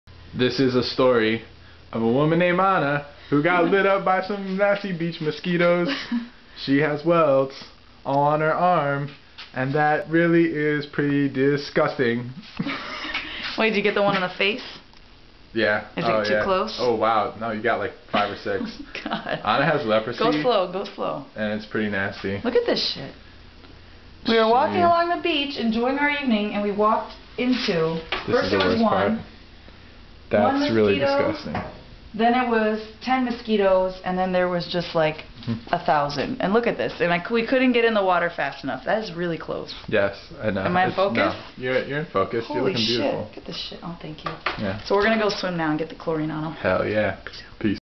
When mosquitos attack-Sept. 2007
Supposedly, Grand Cayman doesn't have mosquitos. Well, I say otherwise. Sept. 2007, me and John Selby at the Sunshine Suites. John is the featured vocalist.